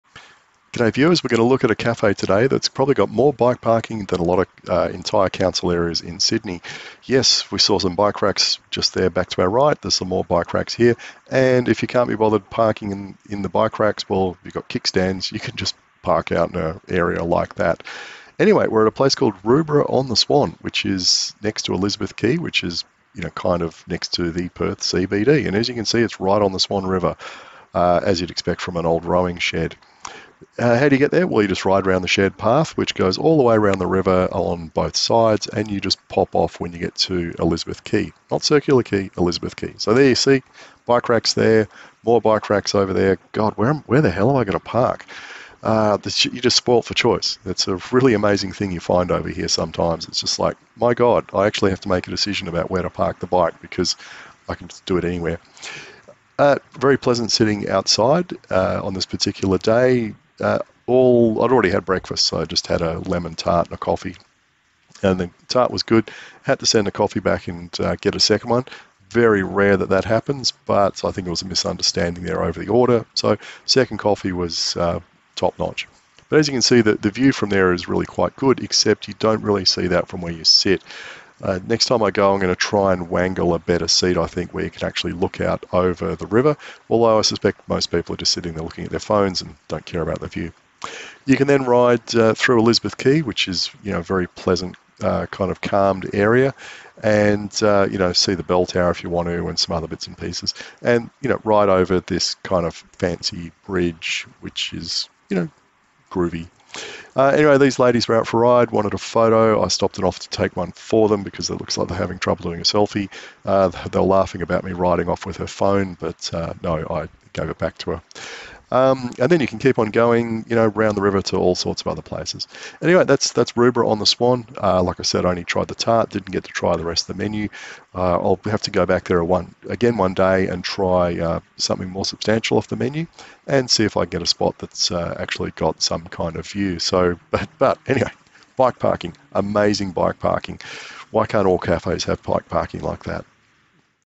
Rubra cafe and the plethora of bike parking
I swear this one cafe has more bike parking available than the entirety of some NSW council areas.